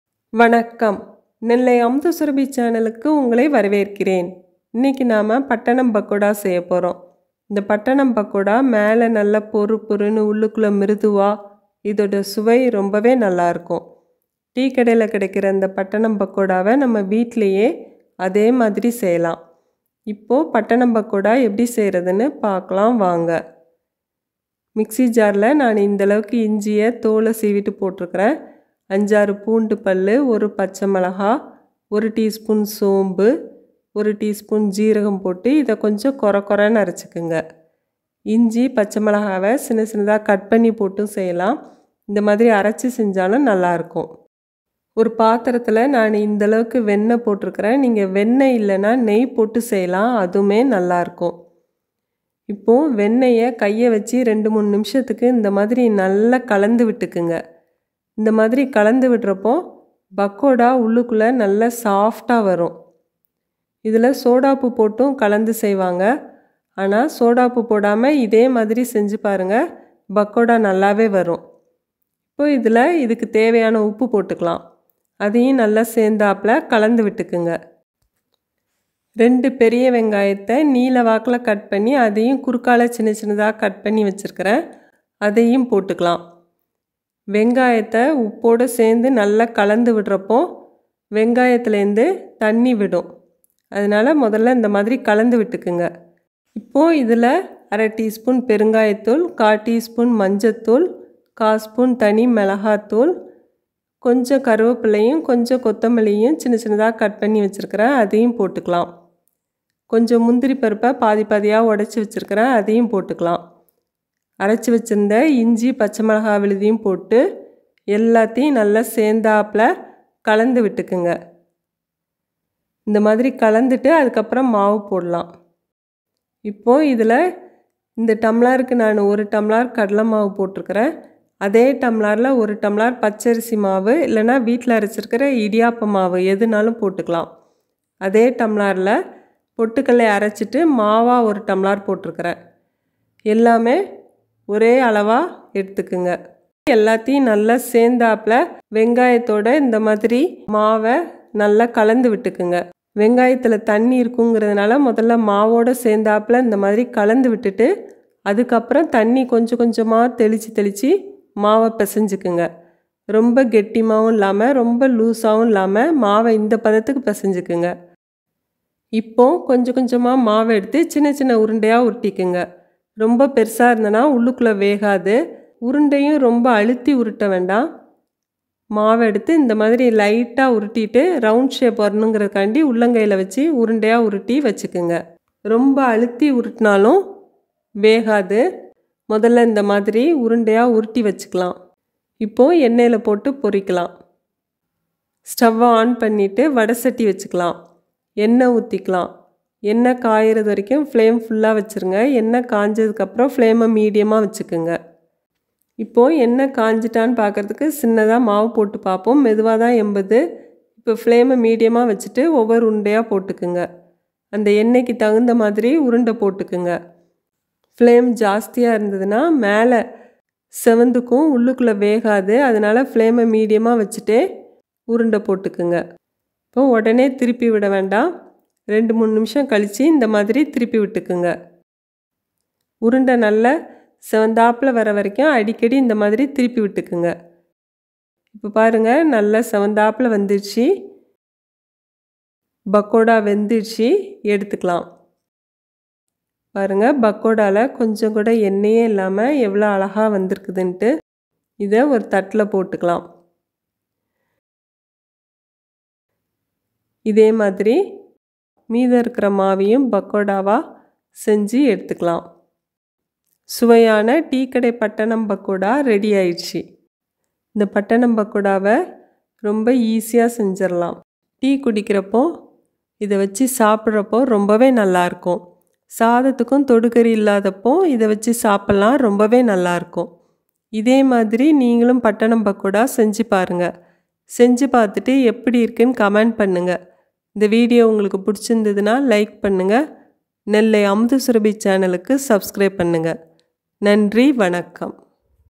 பட்டணம் பக்கோடா/Pattanam Pakoda in Tamil/Medhu Pakoda in Tamil/Medhu Bonda in Tamil/Pakoda Recipe
Welcome to Nellai Amudha Surabi Channel.
This video is about how to prepare pattanam pakoda in tamil.

Try this easy and tasty pattanam pakoda recipe. Share this recipe to your friends and relatives. Also share your feedback about this recipe in comment box.

Recipe 118:Pattanam Pakoda